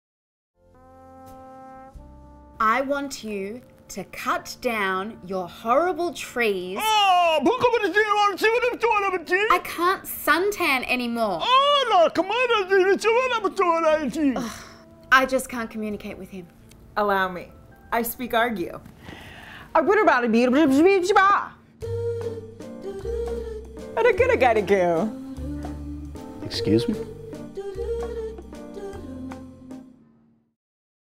30 Second Mediation PSA: I Speak Argue
With court closures, funding crises and delays of justice it's great to know that there is another way to settle disputes--mediation.  Mediation is a dispute resolution process for resolving personal, commercial, government and community disputes that keeps cases out of our beleaguered court system.

This 30 second PSA spot briefly and humorously illustrates the benefits of mediation and provides your viewers with an online resource where they can learn more.